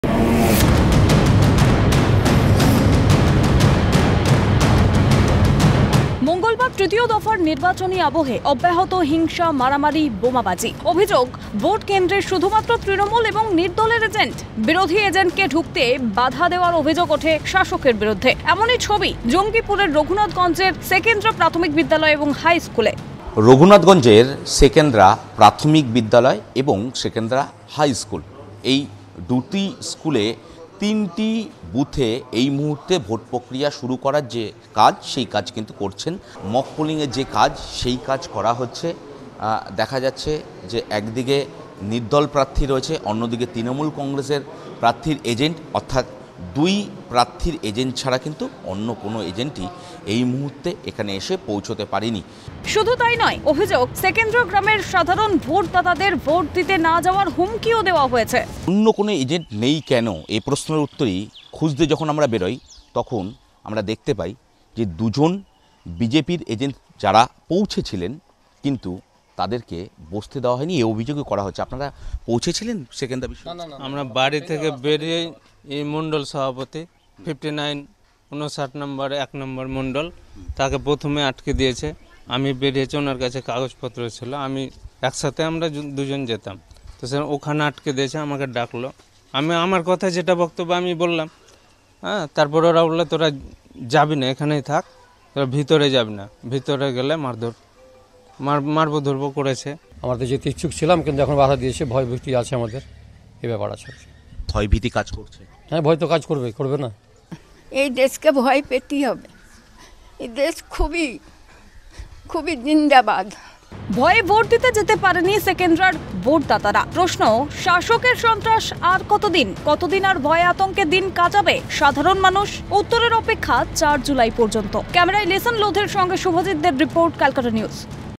ভোটকেন্দ্রে শুধুমাত্র তৃণমূল ও নির্দলের এজেন্ট!
ভোটকেন্দ্রে শুধুমাত্র তৃণমূল ও নির্দলের এজেন্ট! বিজেপির এজেন্টকে ঢুকতে বাধা দেওয়ার অভিযোগ। কাঠগড়ায় সেই তৃণমূল। ঘটনা জঙ্গিপুরের রঘুনাথগঞ্জের সেকেন্দ্রা  প্রাথমিক বিদ্যালয় ও হাই স্কুলে।